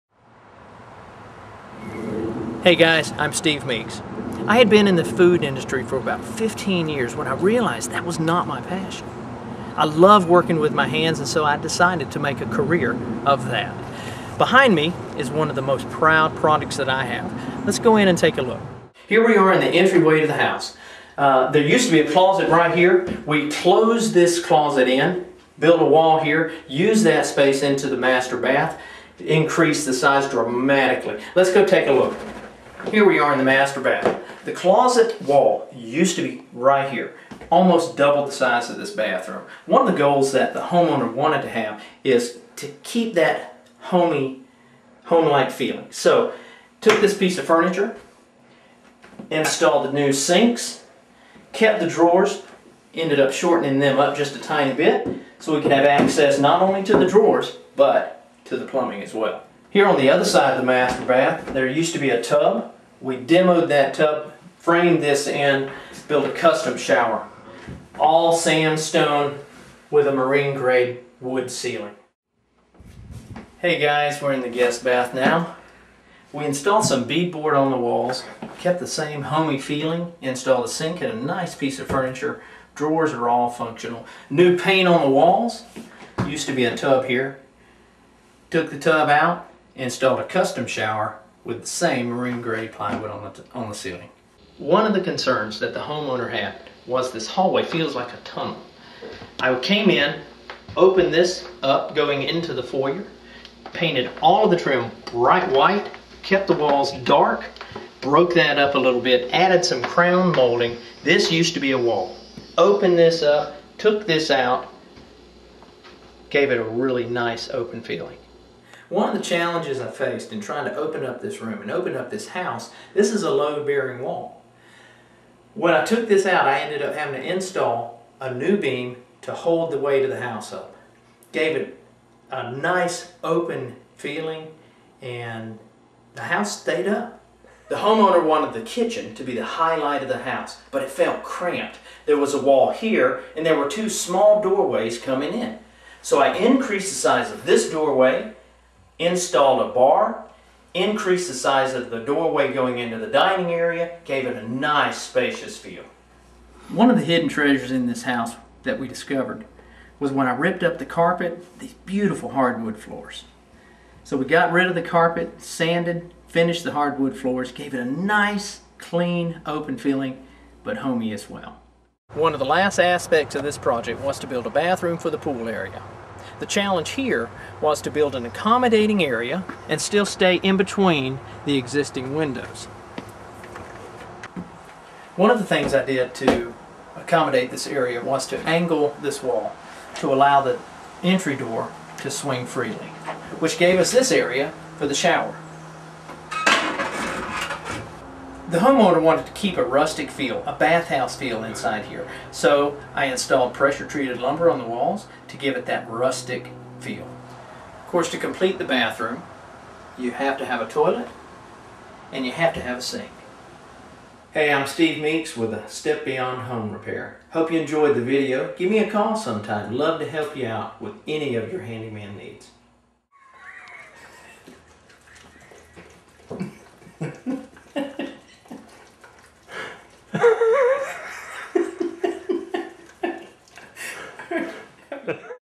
Step Beyond Home Repair
Steve Meeks showcases one of his favorite projects